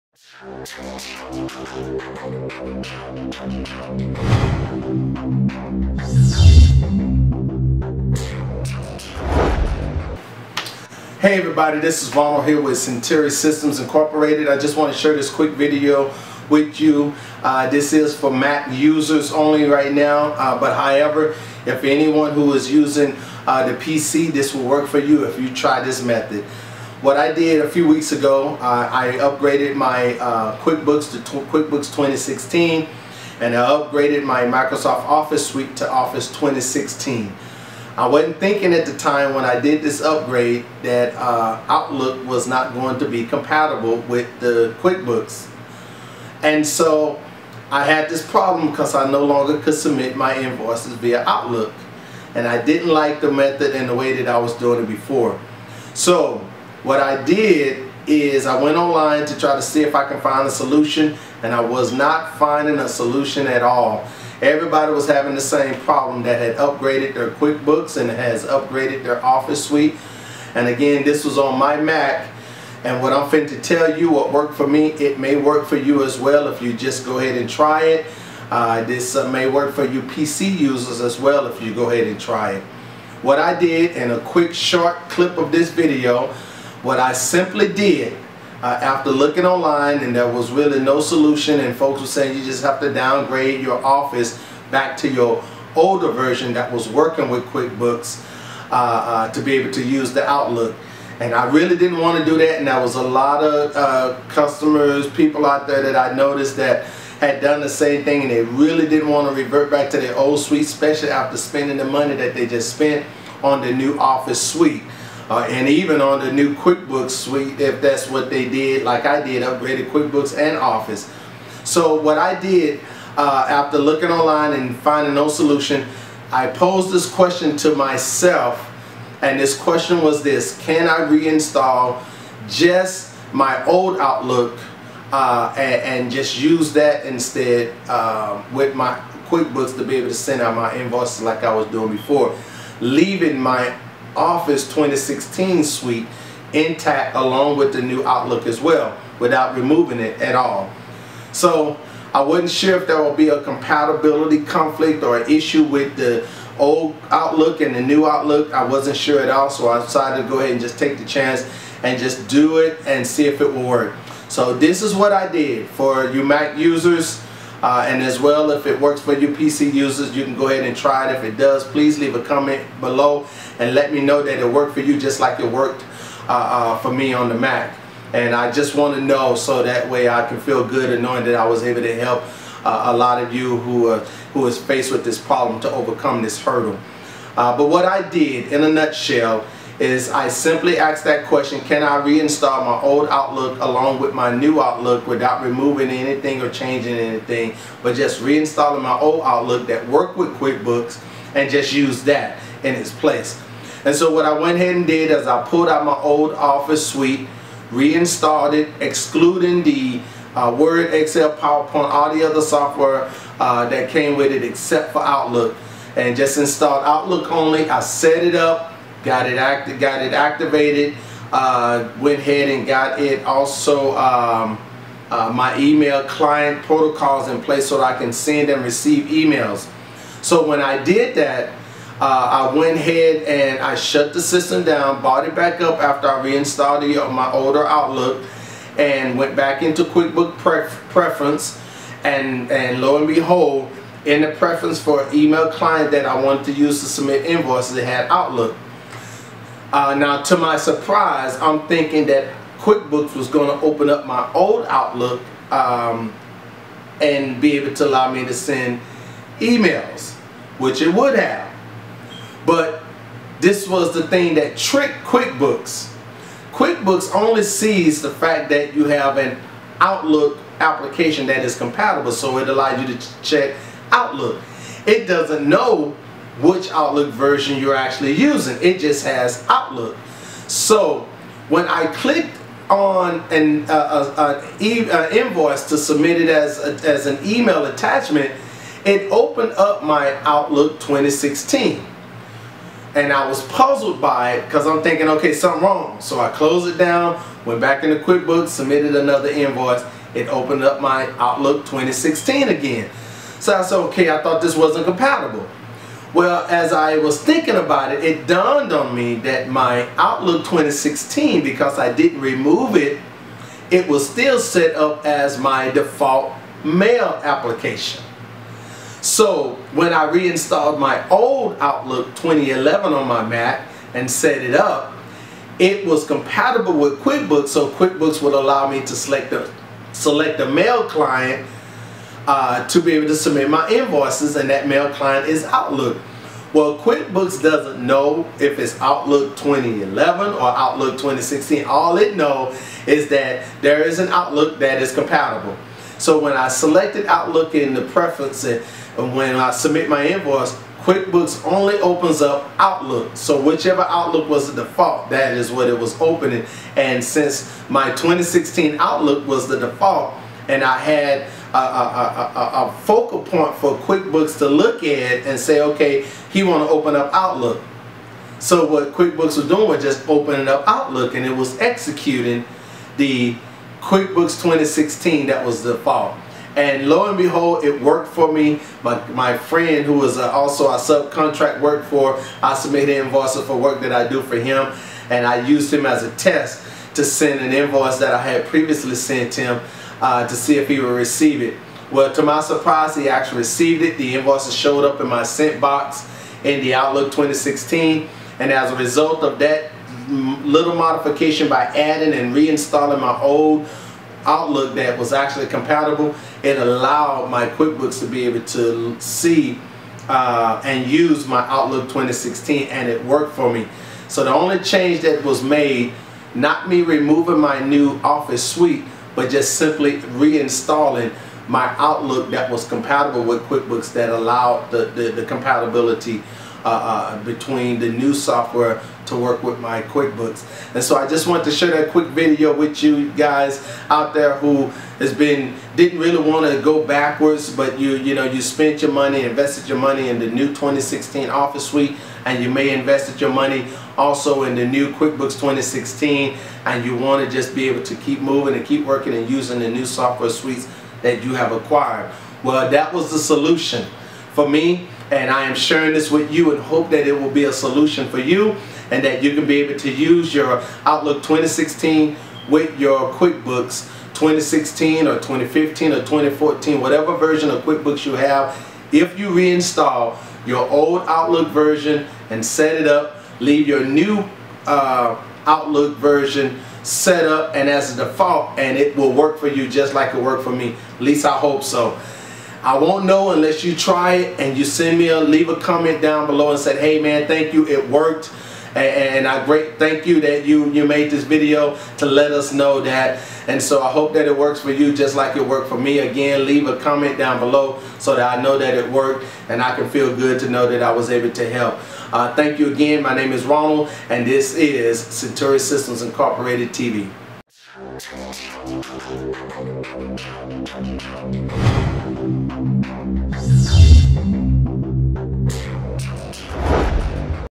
Quickbooks and Office 2016
How I got my outlook 2016 to work with my QuickBooks 2016